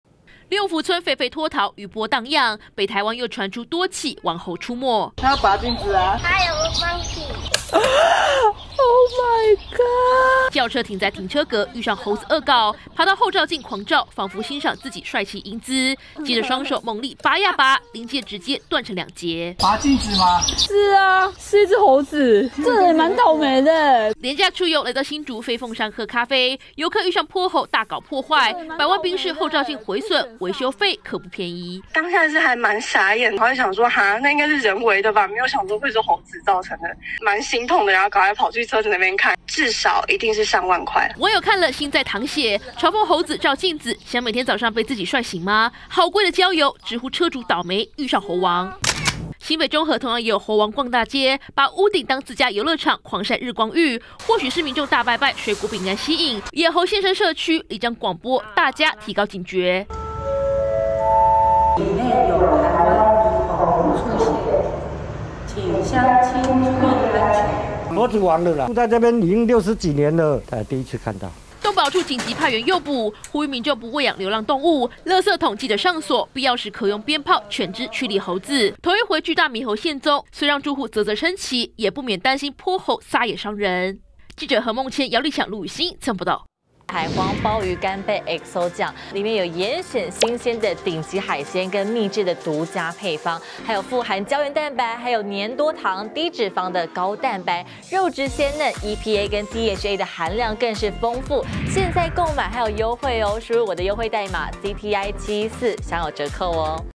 【每日必看】車主心淌血! 潑猴跳車拆後照鏡 維修估花上萬 20230404@中天新聞CtiNews
民眾清明連假
在新竹-飛鳳山景點
喝咖啡，
倒楣遇到頑皮猴，
對著後照鏡狂照，
還大力拔斷鏡子，
逃之夭夭，
愛車被猴子惡搞，
修車費初估上萬元，
車主內心簡直在淌血；
新北中和，
則有隻
半個人高的大獼猴出沒，
悠哉地
曬起日光浴，
卻讓里長
急得廣播要大家出門小心。